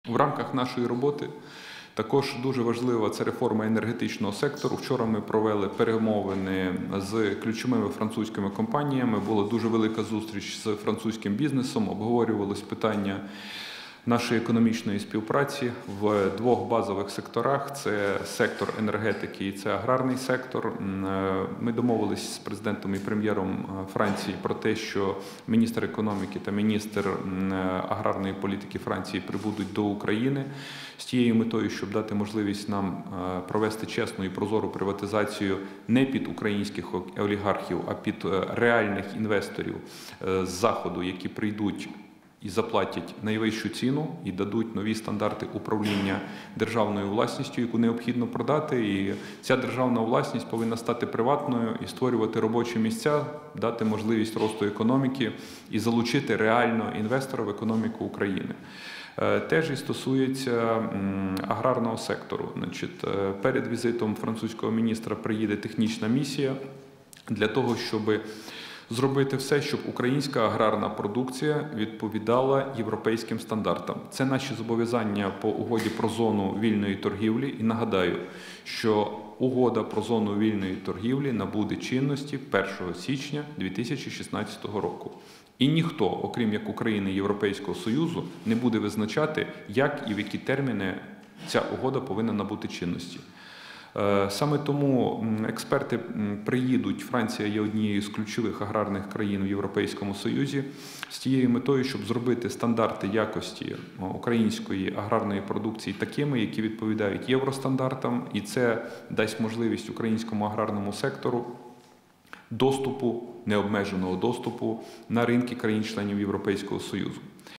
Арсеній Яценюк про приватизацію «Чесна і прозора Не під українських олігархів, а під реальних інвес
«Державна власність повинна стати приватною і створювати робочі місця, дати можливість росту економіки і залучити реального інвестора в економіку України», -   наголосив на засіданні Уряду в четвер, 14 травня, Прем’єр-міністр України, лідер політсили «Народний фронт» Арсеній Яценюк.

Він нагадав, що під час робочого візиту до Парижа 13 травня він мав зустріч із представниками французьких ділових кіл, під час якої обговорювалося питання економічної співпраці України та Франції «у двох базових секторах – енергетиці та аграрному секторі».

Під час зустрічей Глави Уряду України з Президентом Франції Франсуа Олландом та Прем'єр-міністром Франції Мануелем Вальсом було домовлено про прибуття до України Міністра економіки та Міністра аграрної політики Франції – для допомоги в проведенні «чесної і прозорої приватизації не під українських олігархів, а під реальних інвесторів з Заходу, які заплатять найвищу ціну і дадуть нові стандарти управління державною власністю».

«Державна власність повинна стати приватною і створювати робочі місця, дати можливість росту економіки і залучити реального інвестора в економіку України», - підкреслив він.

Щодо співпраці в аграрному секторі Глава Уряду повідомив, що до України прибуде технічна місія, «щоб зробити стандарти якості української аграрної продукції такими, які відповідають євростандартам»: «Це дасть українському аграрному сектору можливість необмеженого доступу на ринки країн-членів Європейського Союзу».

«Це наші зобов’язання по угоді про зону вільної торгівлі, яка набуде чинності 1 січня 2016 року», - наголосив Арсеній Яценюк.

«Ніхто, окрім України і Європейського Союзу, не буде визначати, як і в які терміни Угода про зону вільної торгівлі повинна набути чинності», - наголосив Глава Уряду.

Приєднуйся!